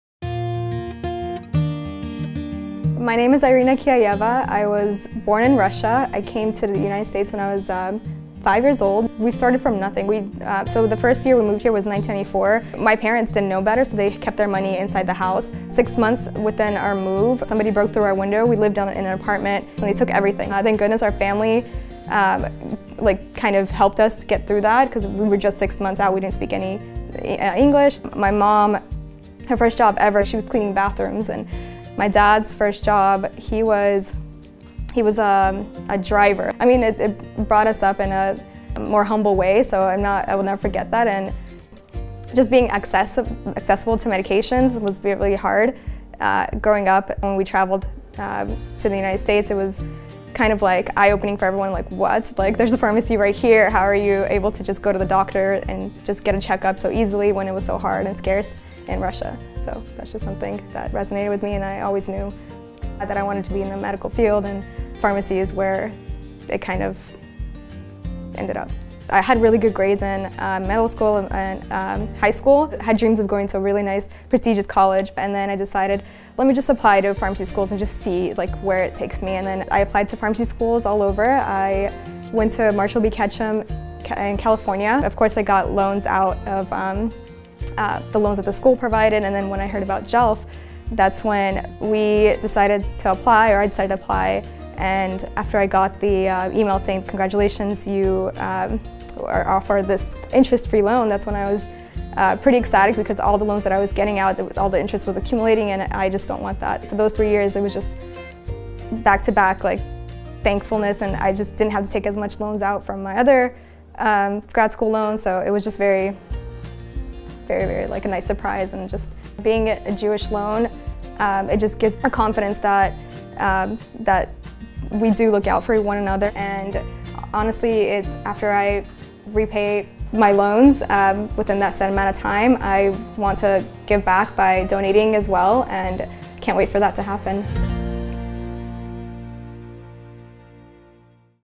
Irena K. - My JELF Story
Irene K. moved to the U.S. with her family as a little girl. Today, as a pharmacist, she is repaying her loans to JELF and as she puts it, has experienced 'back-to-back thankfulness!" for what the loans allowed her to pursue.